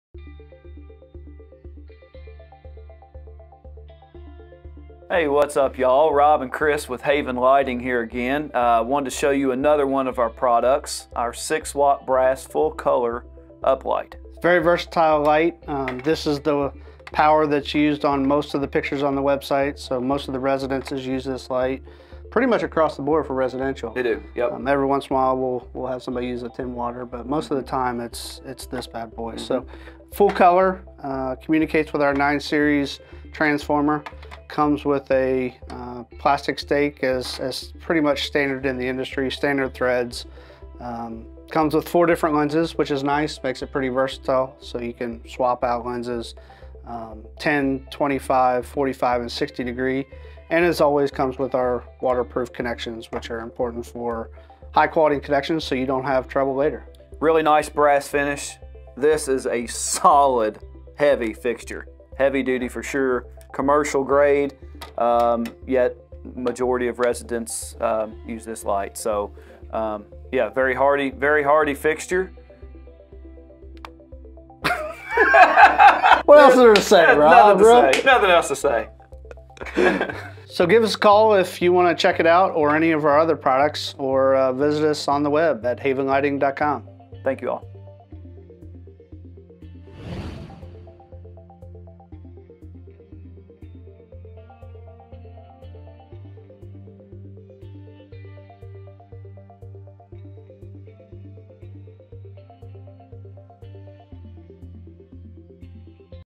Light Talk with Rob and Chris | Episode 7 | 6W Brass Up Light
Join us as Sales Guy Rob and Founder Chris talk about our innovative product line, which includes brass up lights. From classic landscape fixtures we've all come to know and trust, to new ideas put forth by the Haven team to push the industry forward, Light Talk is the perfect place to get an overview of all our products.

In this episode, Chris and Rob introduce Haven’s brass up light fixture, a versatile Haven product that’s one of our most popular for residential properties (check out the product link in the comments). This full-color light works with the 9 Series transformer (see link in comments) to turn your property into a vibrant showpiece.

Here’re the video’s highlights:

- Haven provides a standard plastic stake, which you can upgrade, with all brass up lights.
- For even more versatile lighting options, our up lights come with four different lenses: 10°, 25°, 45° and 60°.
- Our brass up lights come with waterproof wire nuts for the best connection.
- The light’s solid construction and finish make it a heavy-duty, commercial-grade fixture perfect for standard landscapes and lawns.

Learn more about the brass up light fixture and see how it can transform your property at Haven Lighting (see link in the comments section below).